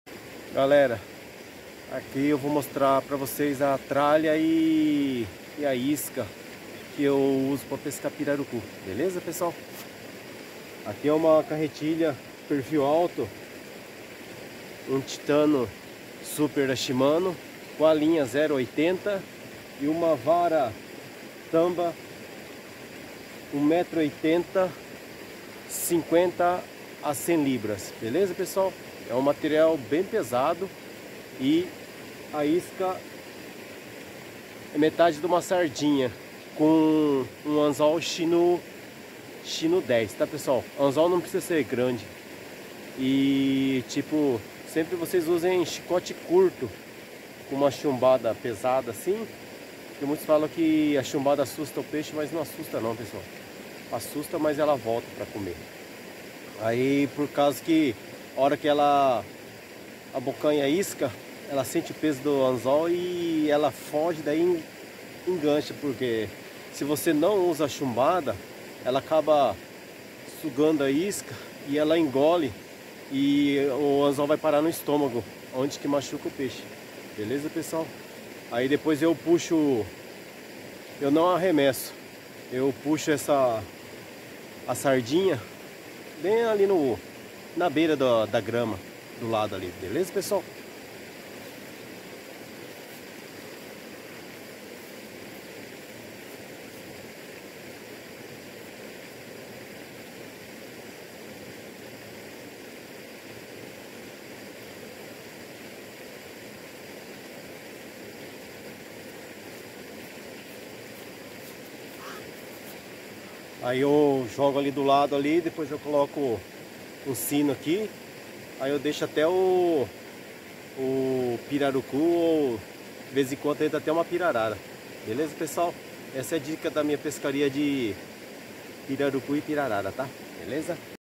Dicas de como preparar e pescar pirarucu em pesqueiro#
Como eu preparo a minha tralha de pesca para pescar pirarucu e seus cuidados para o peixe não engolir o anzol.
O grande segredo é colocar uma chumbada de uns 30 a 50 gramas acima do girador.
E abaixo do girador colocar um chicote curto de uns 30cm para o pirarucu não engolir o anzol.
Imagine o peixe engolir o anzol no estomago e ser  fisgado deve ser uma imensa dor  e depois tem o perigo do anzol rasgar o estômago e morrer após alguns dias.